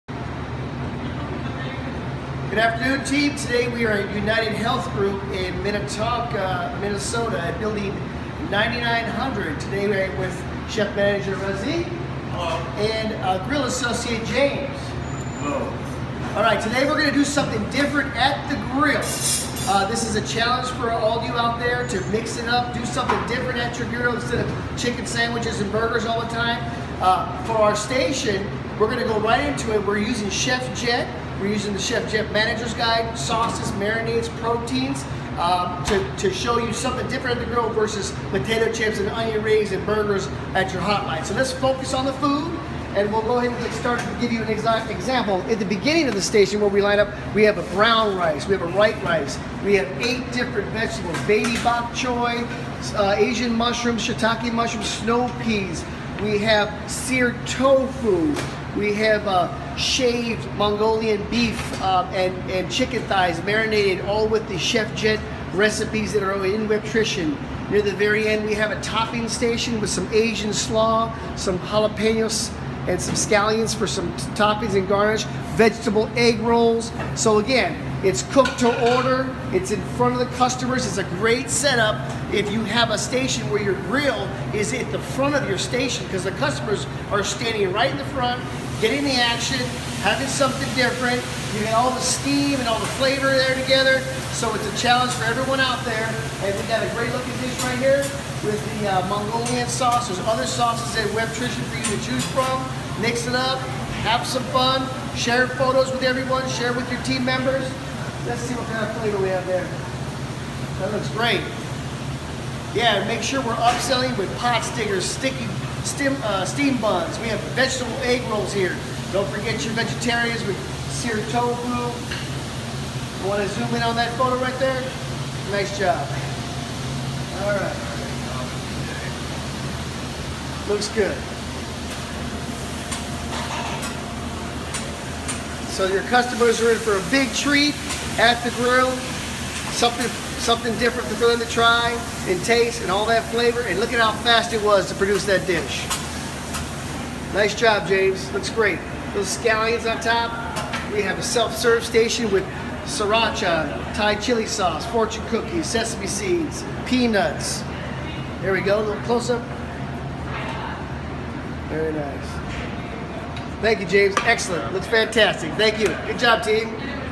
Mongolian Grill
Quick Stir Fry off the grill station (in place of burgers and chicken sandwiches).  Laid out for quick sear, sounds and smells of asian spices, garlic and ginger.  Great option to rotate out a tired old grill setup...